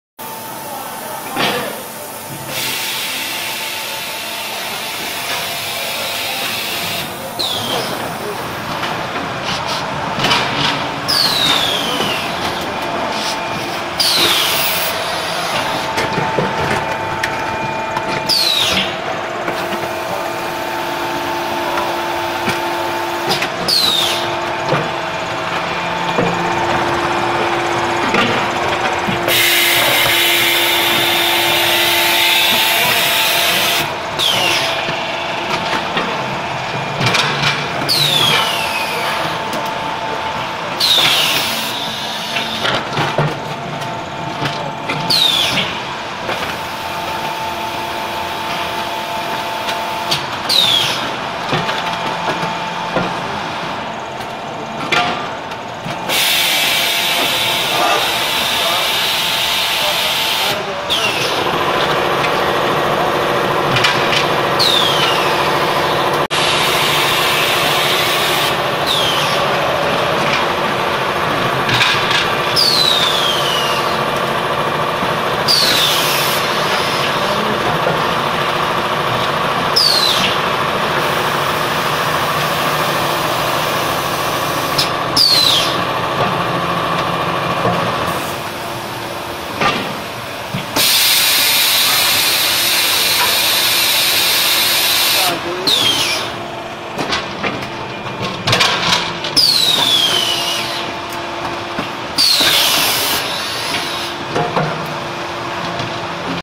Double layers terrazzo floor tile making machine,stone press machine,artificial stone machine,
Double layers terrazzo floor tile machine is for thick tile., which makes the tile has perfect surface and saving the raw materials cost.
Skype:ediezhai